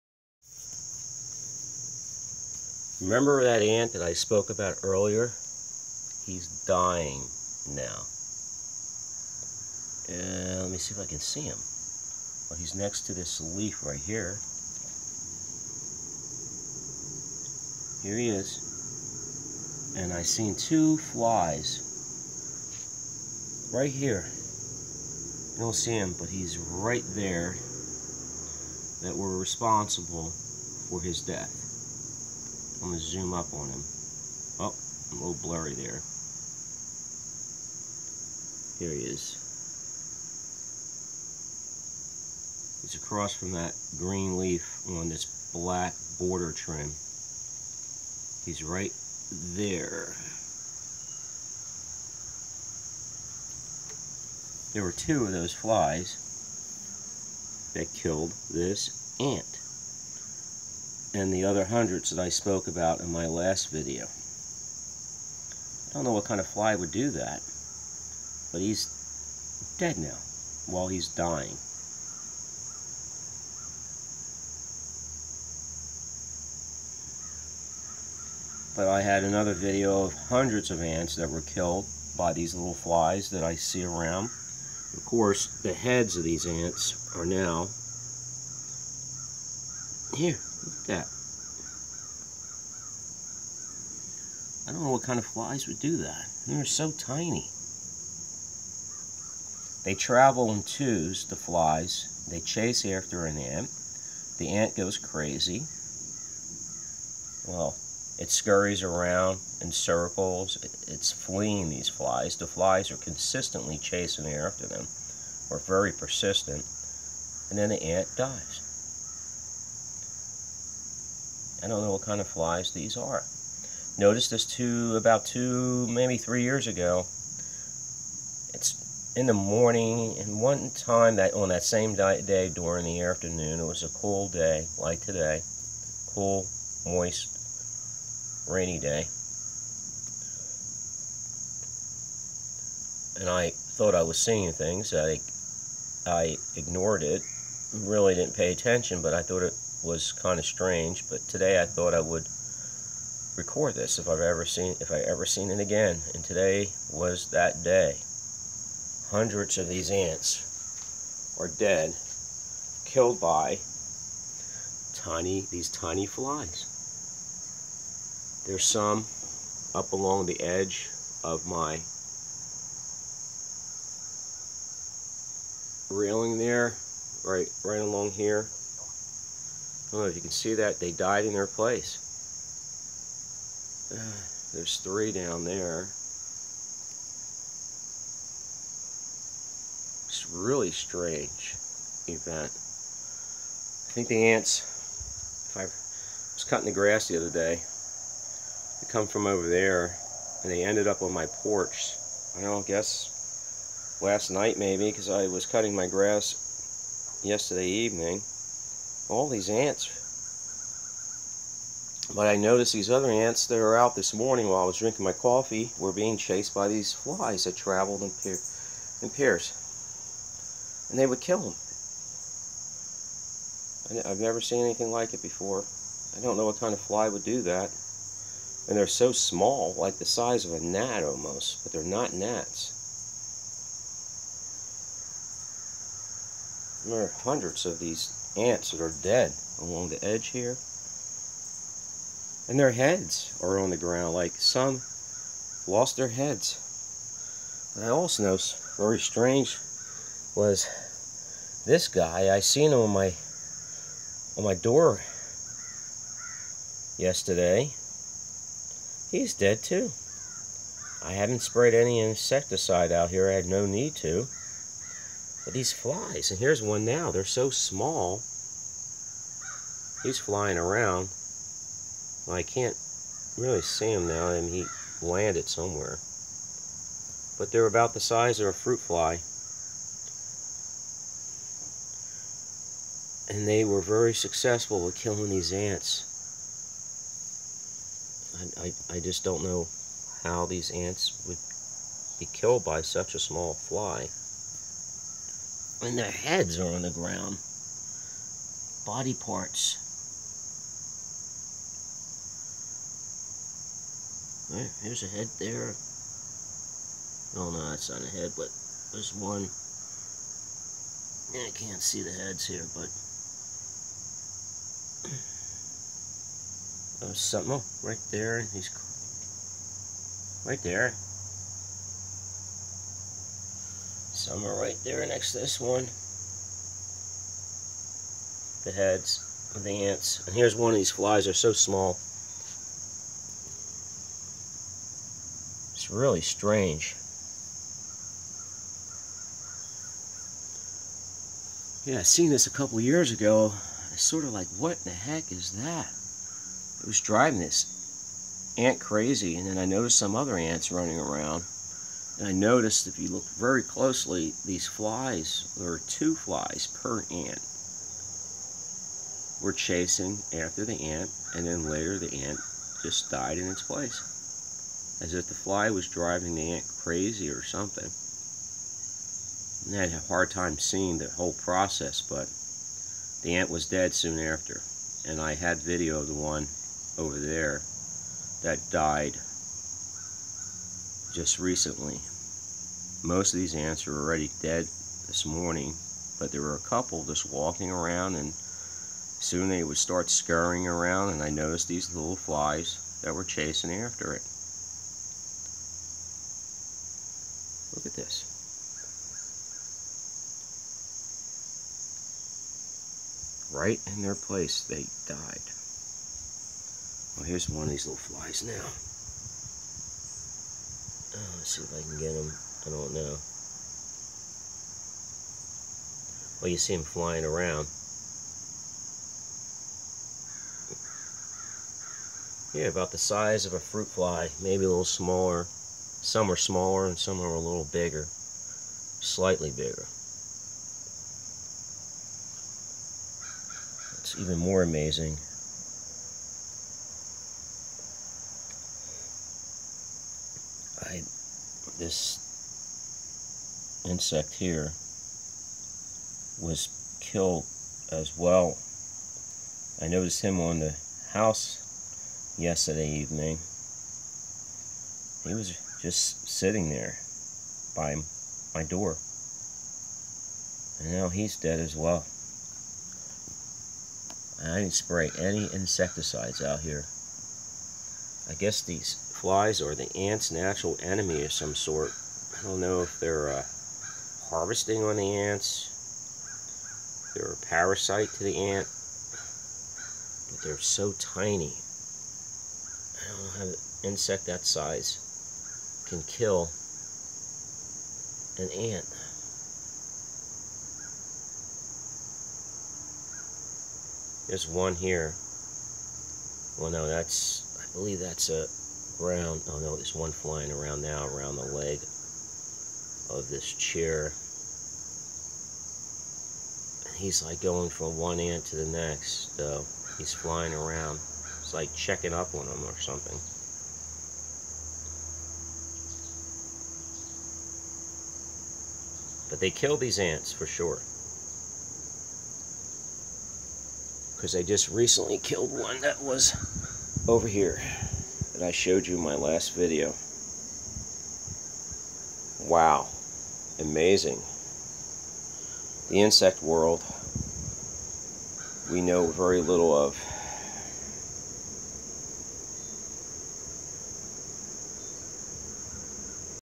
Carpenter Ants killed by small flies similar to fruit flies. Strange. - Part 2
A video of the aftermath of what these flying insects did to many Carpenter Ants.  Amazing and strange.  What type of fly the size of a fruit fly can easily kill a Carpenter Ant 20 times its size?  I have no idea so I recorded the aftermath of the attack and later an attack on a Carpenter Ant by this unusual species of flying insects. Maybe someone out there in the world knows what type of insect the size of a fruit fly can do this to an Carpenter Ant.